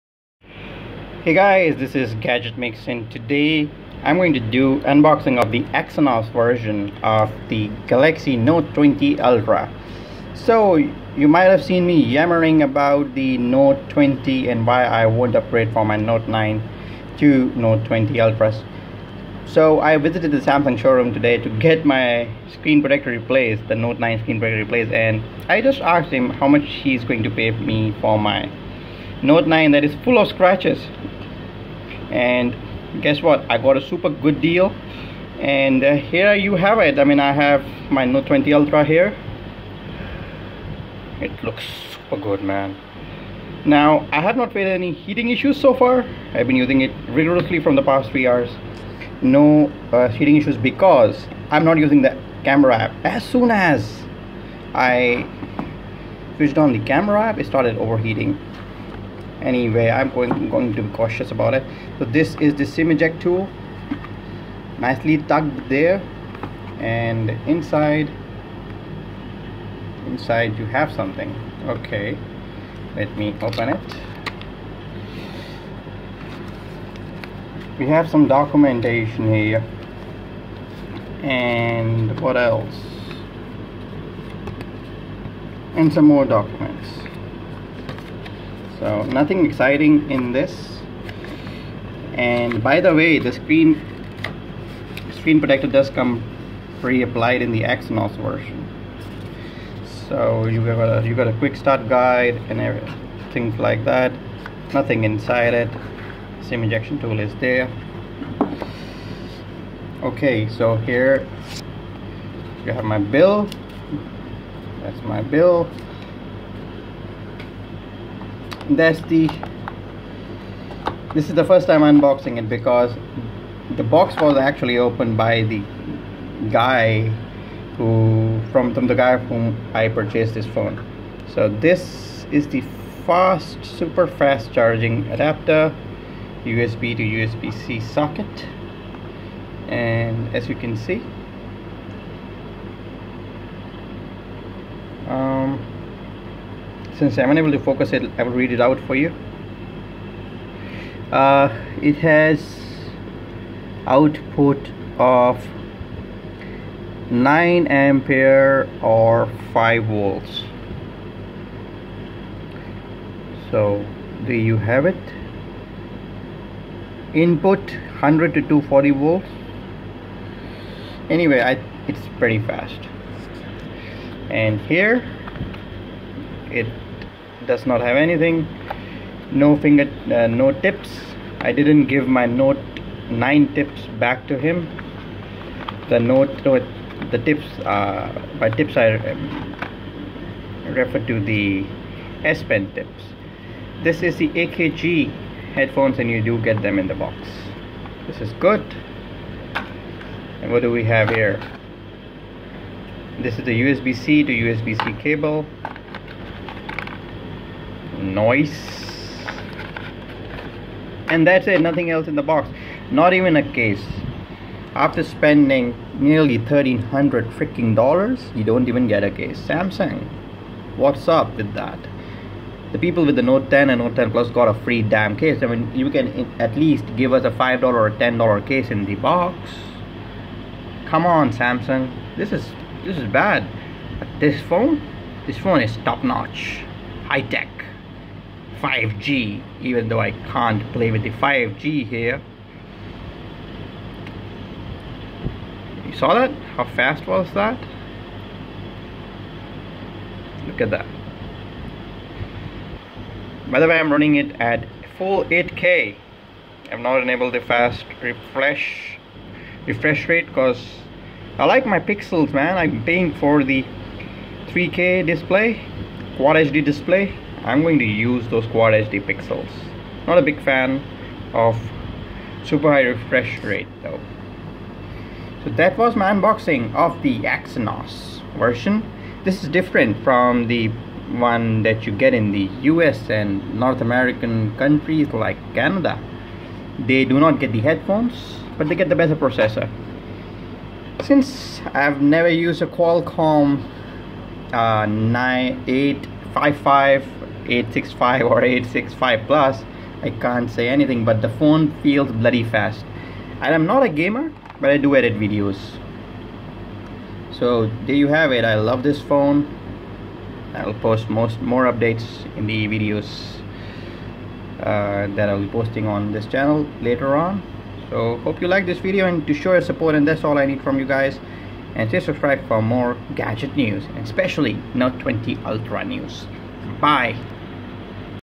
Exynos Samsung Galaxy Note 20 Ultra Unboxing -Earphones included but no case in Note 20 Ultra retail
This is unboxing of retail version of Samsung Galaxy Note 20 Ultra 5G EXYNOS variant.